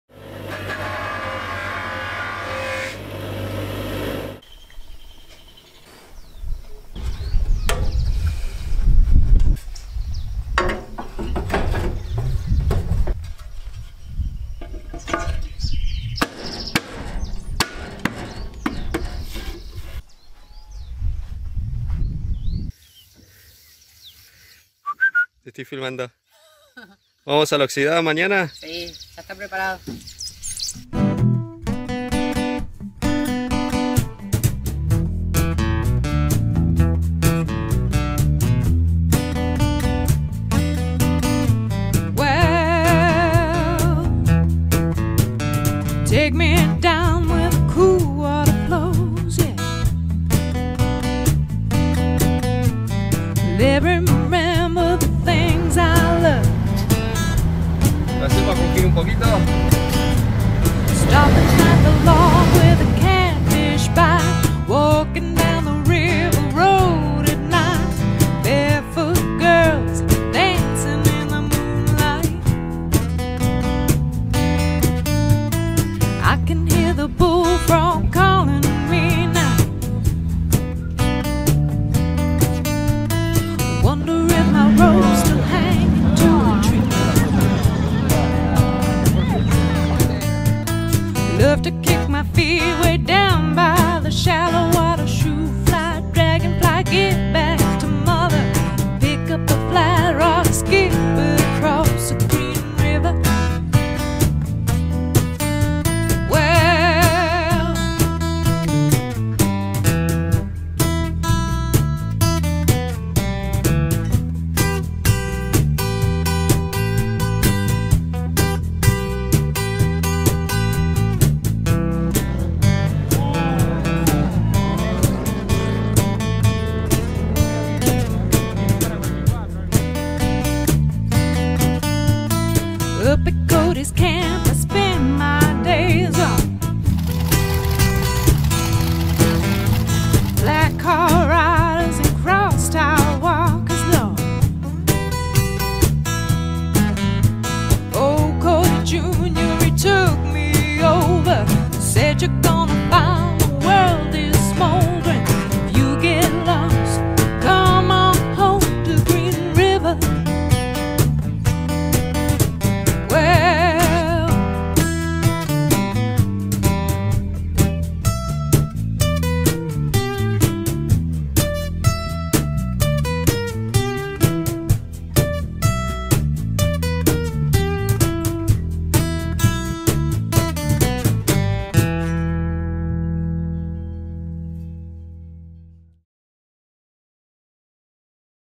Juntada la oxidada 2022 - Bs. As. Argentina
Preparando el auto y disfrutando de la juntada "la oxidada 2022".
Muchas horas dedicadas a restaurarlo, con mucho amor. Como prácticamente todos los que llevamos autos clásicos a este tipo de encuentros, donde exponemos nuestro arte de mecánico; chapista; electricista; tapicero y pintor de la vieja escuela.